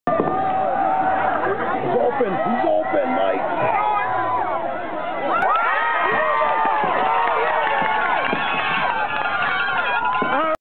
25 Yard Reception
Victor Bodrie, JV. Year, 25 yard reception